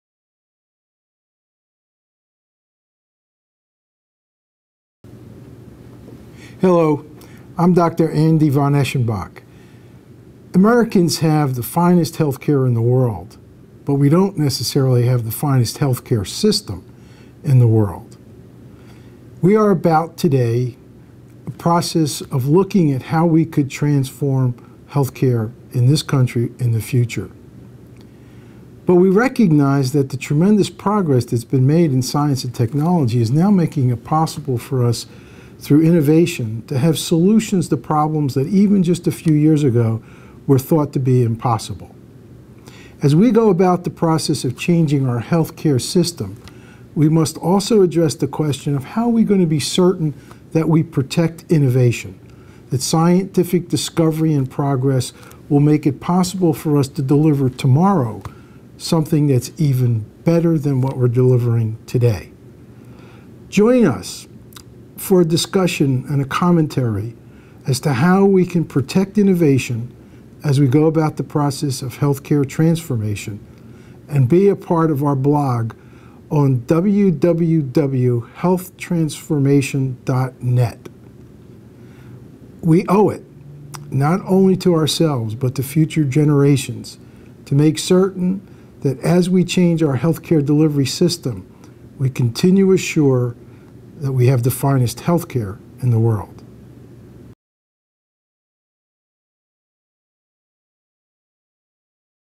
Dr. Andy von Eschenbach on Accelerating Scientific Discovery and Innovation
Learn how a 21st Century Personalized Health System impacts you -- Accelerating Scientific Discovery and Innovation (including discovery, development and delivery of solutions that create longer, healthier lives at lower overall cost) blog.